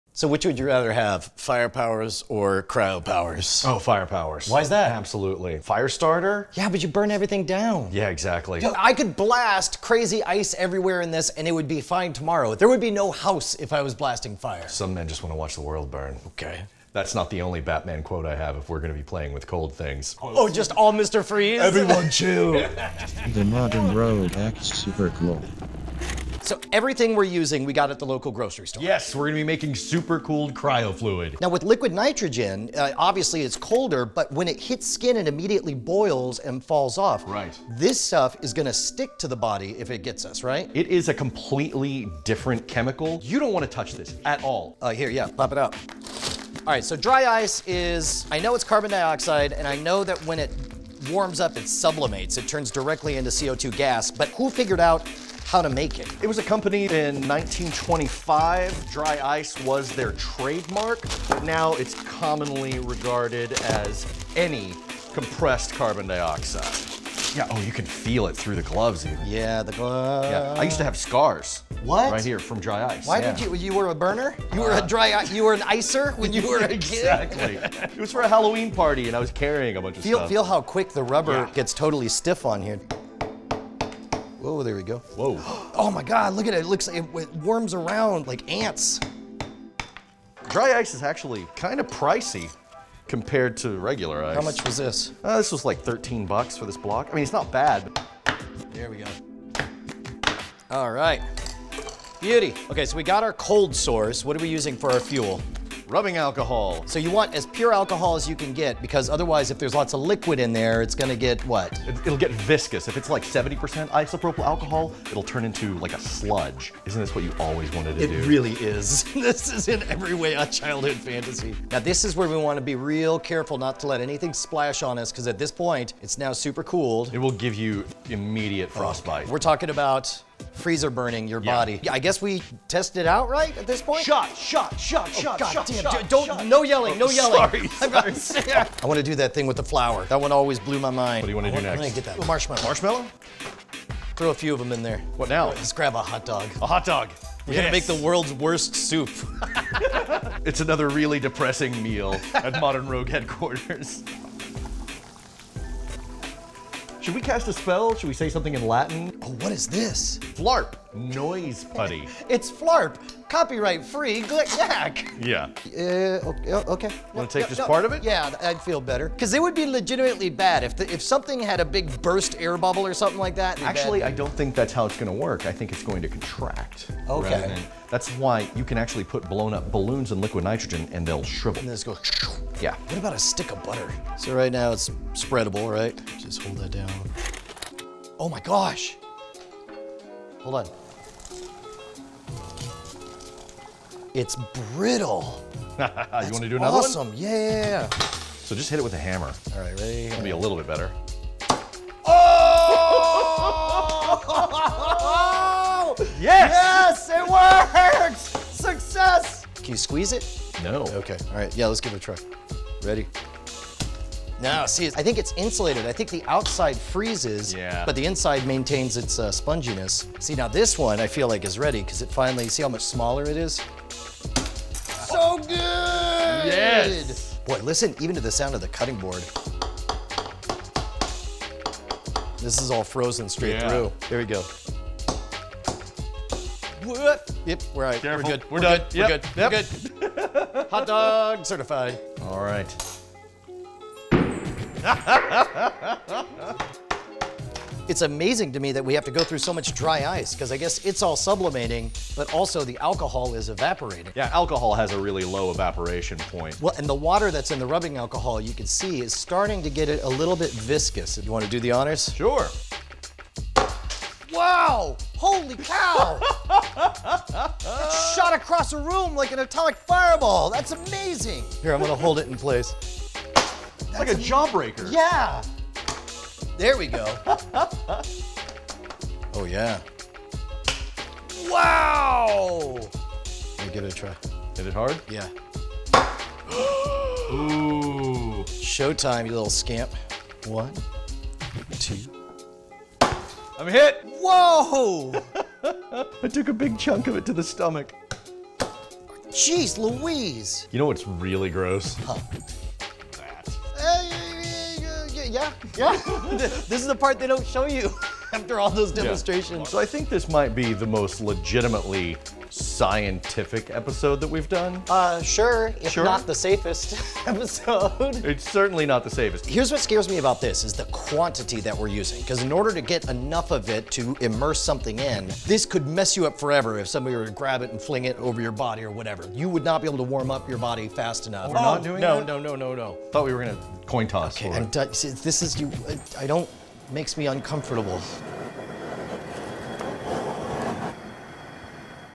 How to Make Supercooled Cryofluid (4K Remastered Version)
Brian and Jason make cryogenic fluid by supercooling isopropyl alcohol with dry ice, and are obligated to freeze and shatter things.

If for whatever reason you decide to make some psycho soup for yourself, take safety precautions and understand that this is some dangerous stuff. You definitely don't want to consume anything frozen with this stuff, you don't want to be anywhere near touching this stuff, and at the end of the day you probably just don't even want to make it.